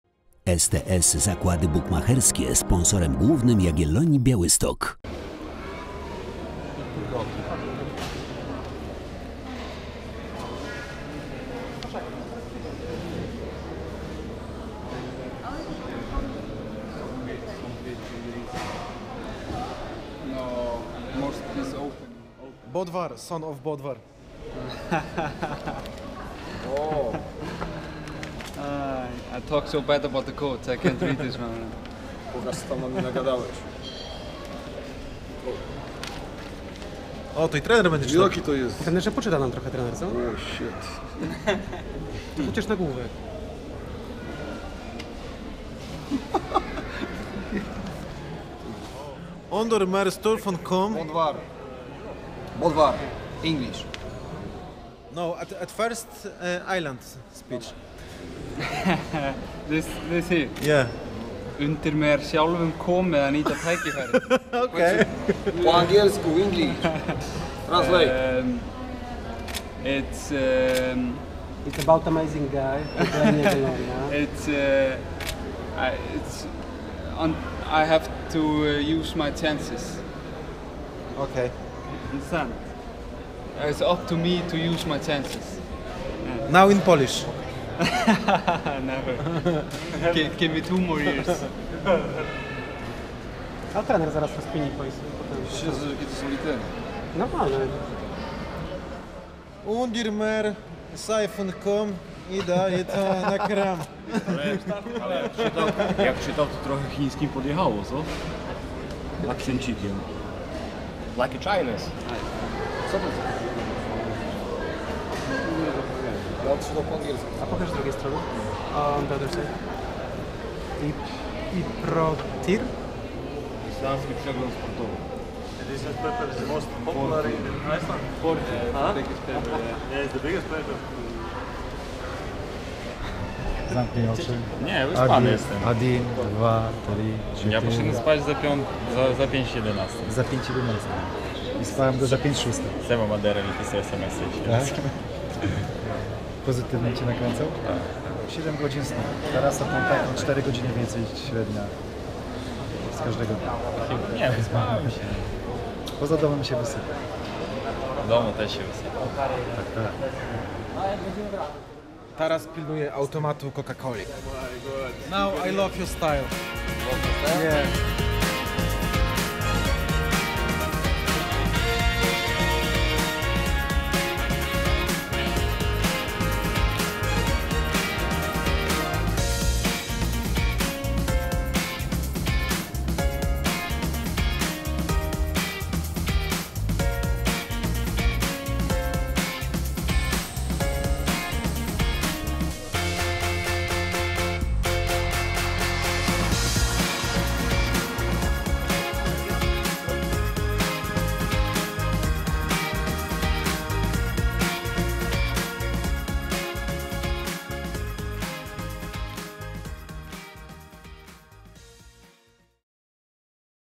Jaga przybyła do Portugalii
W czwartek wieczorem piłkarze Jagiellonii Białystok powalczą o awans do III rundy eliminacji Ligi Europy. W wyjazdowej rywalizacji z portugalskim Rio Ave podopieczni Ireneusza Mamrota postarają się obronić jednobramkową przewagę z pierwszego spotkania przy Słonecznej.